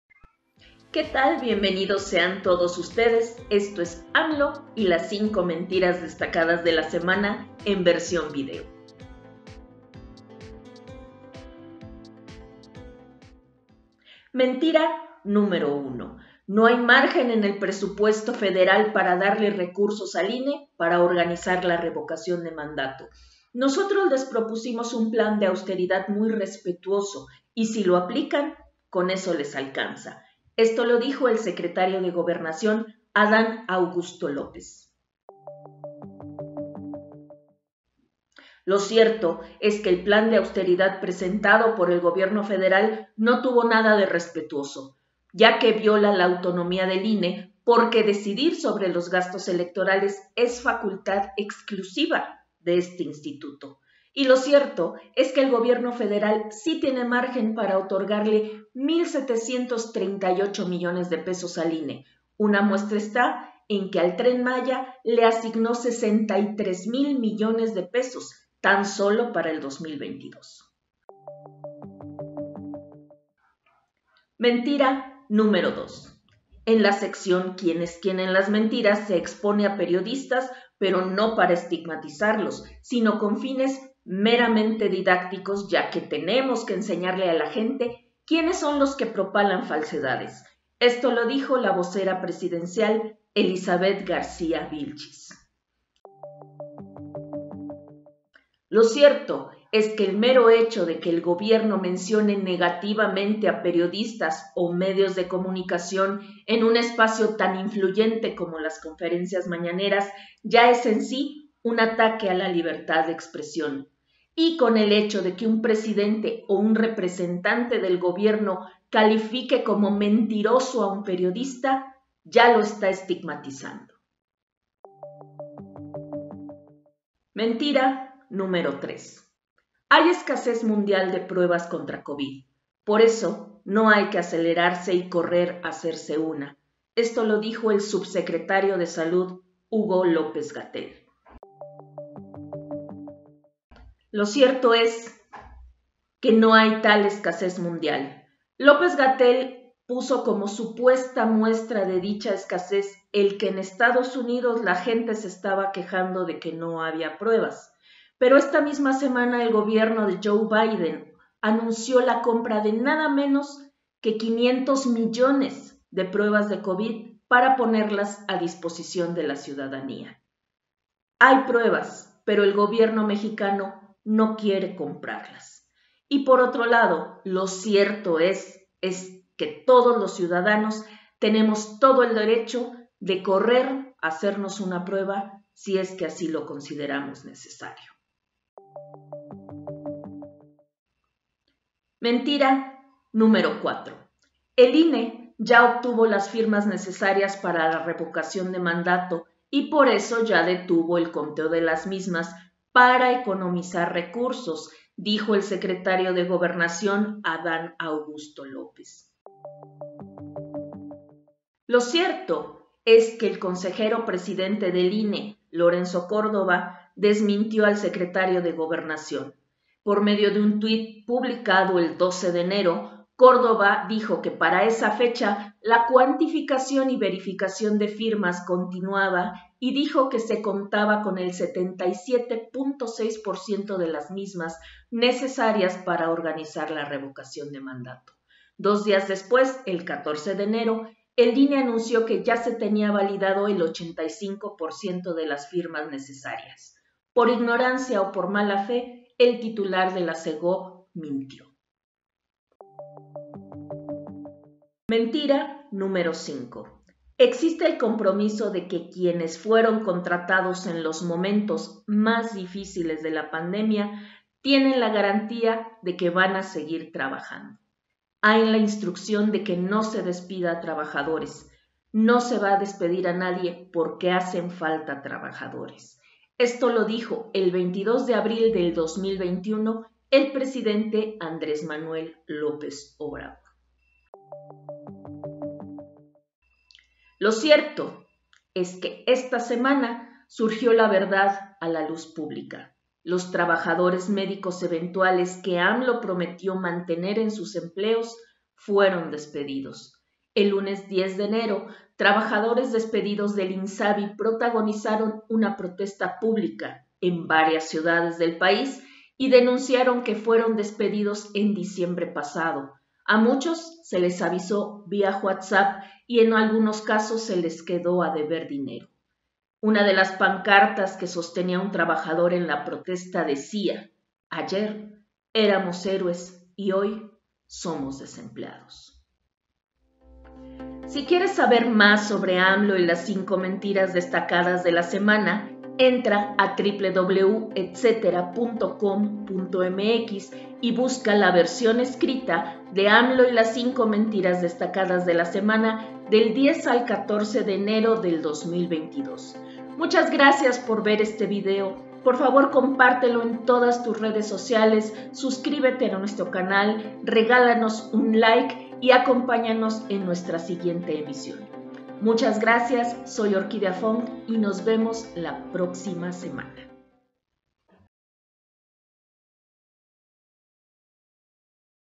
Dice el gobierno que no tiene dinero para darle al INE. Las 5 Mentiras de la Semana.
Dice el gobierno que no le alcanza para darle al INE recursos para la revocación de mandato. Y eso que es una orden que tiene que cumplir. Las 5 Mentiras de la Semana, por Orquídea Fong.
Donaciones:
Cuenta Santander Marco Levario Turcott
Tarjeta:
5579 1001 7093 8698
Clabe:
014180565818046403